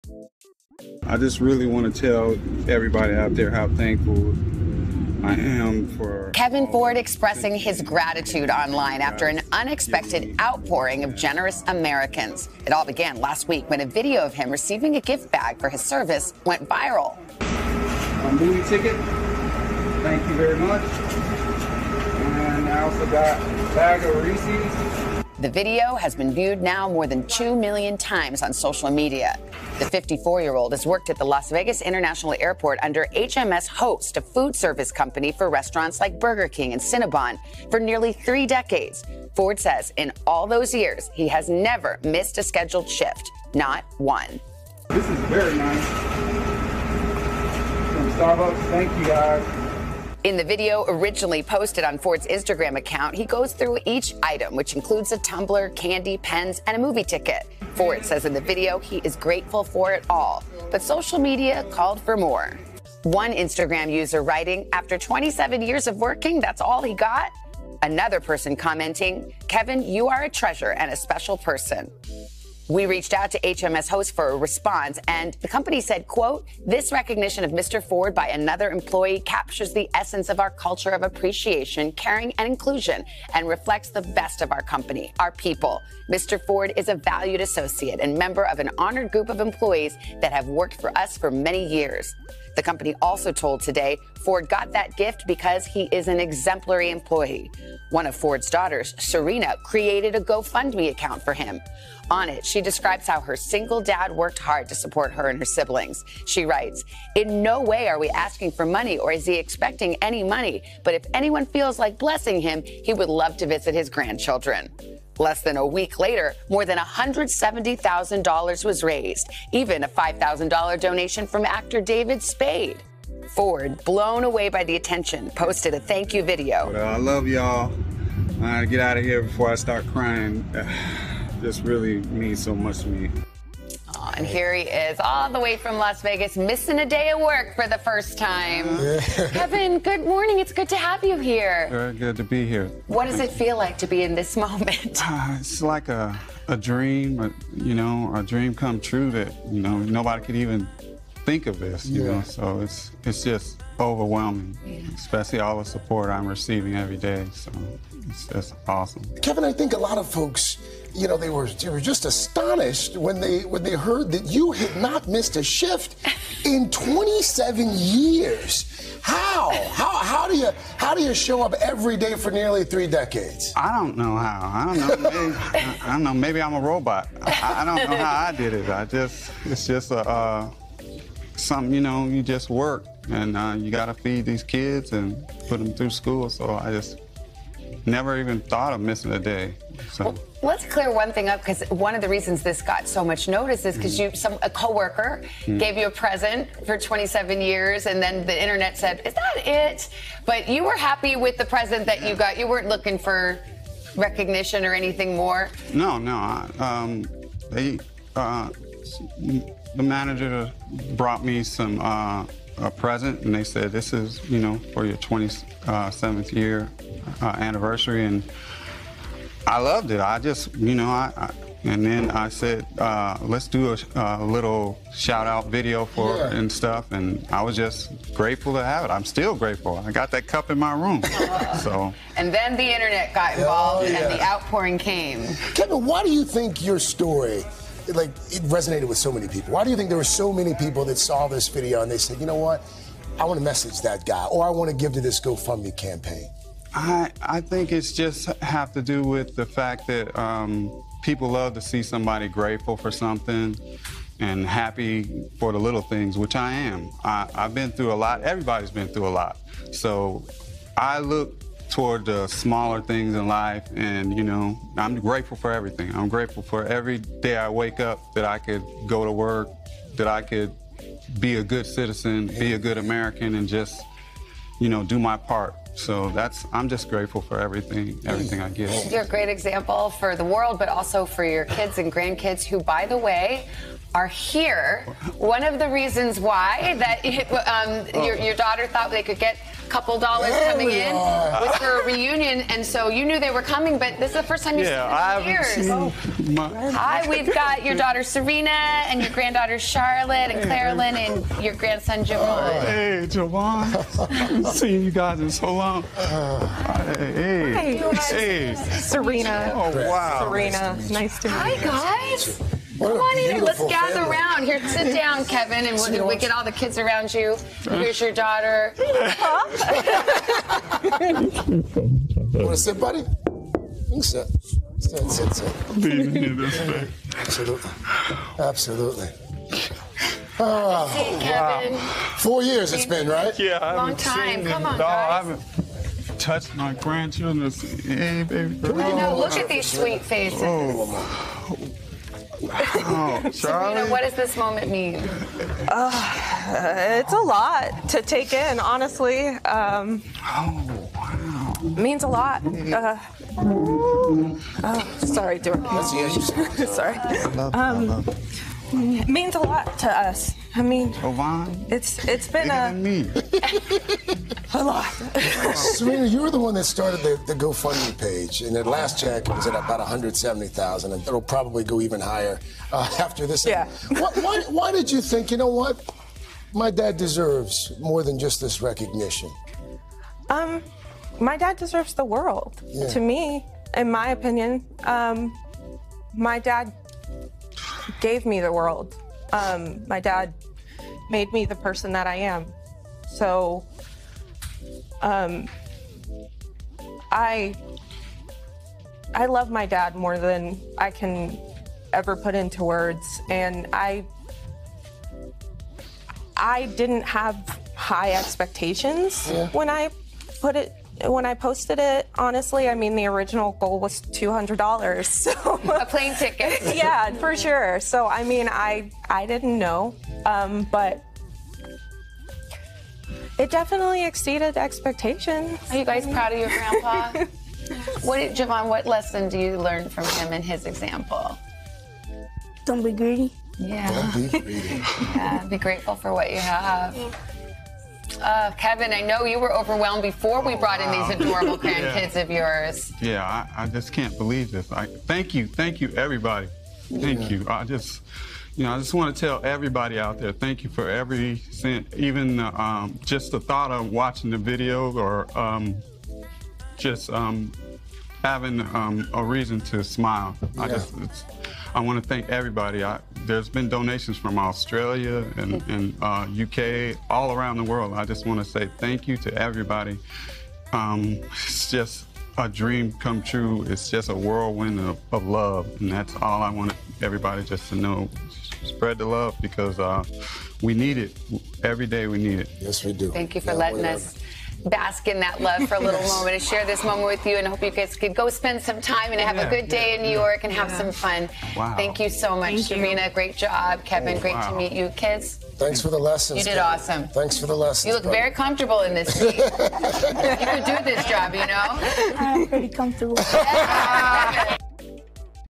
INSPIRATION!!!! GoFundMe Changed K27Y Life Forever!!! One Year Ago...
THANK YOU ALL!!!
You Have Changed My Life and the Lives of My Children, Grandchildren and Generations FOREVER!!!
KEVIN-27-YEARS BK
Grateful for the Little Things...

NOW THIS!!!

Ashley Banfield...

Kevin-27-Years FACEBOOK...

KEVIN-27-YEARS BK Etsy...

Thank You and God Bless You and Yours... LLP Kevin Ford